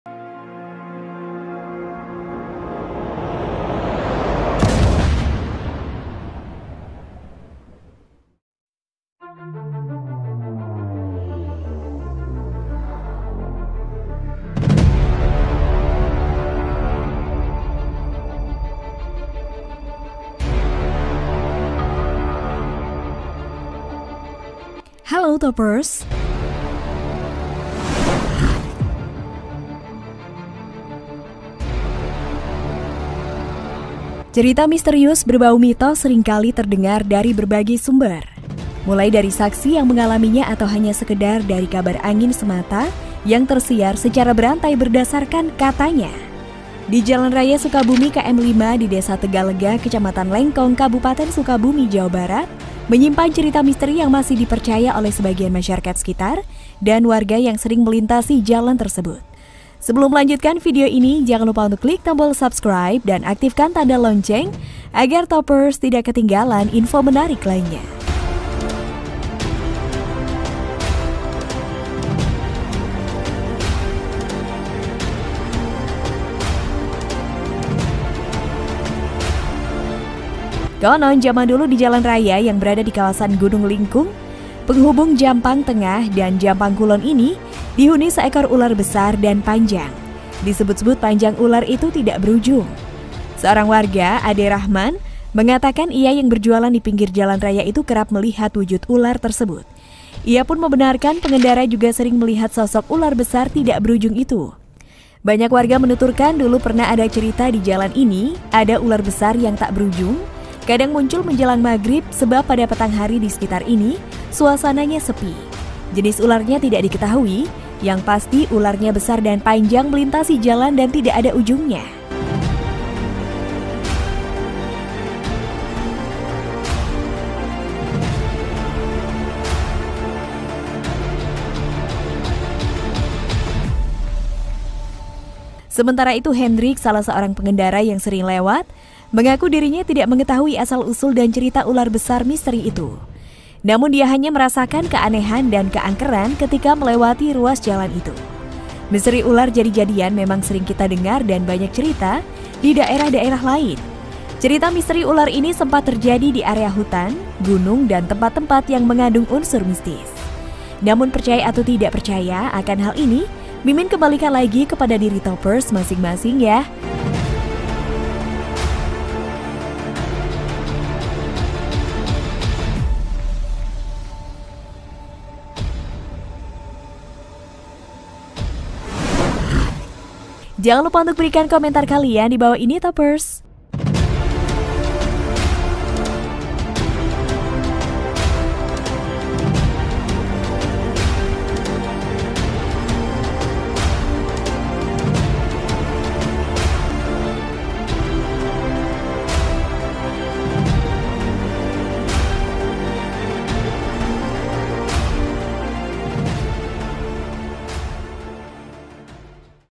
Misteri Penampakan Ular Raksasa Dengan Panjang Tak Berujung di Jalan Sukabumi KM.5 !! BUKAN RAHASIA
Cerita misterius berbau mitos sering kali terdengar dari berbagai sumber. Mulai dari saksi yang mengalaminya atau hanya sekedar dari kabar angin semata yang tersiar secara berantai berdasarkan "Katanya".

Di jalan raya Sukabumi KM 5 di Desa Tegalega, Kecamatan Lengkong, Kabupaten Sukabumi, Jawa Barat menyimpan cerita misteri yang masih dipercaya oleh sebagian masyarakat sekitar dan warga yang sering melintasi jalan tersebut.

Konon, zaman dulu di jalan raya yang berada di kawasan Gunung Lingkung penghubung Jampang Tengah dan Jampang Kulon ini dihuni seekor ular besar dan panjang. Disebut-sebut panjang ular itu tidak berujung.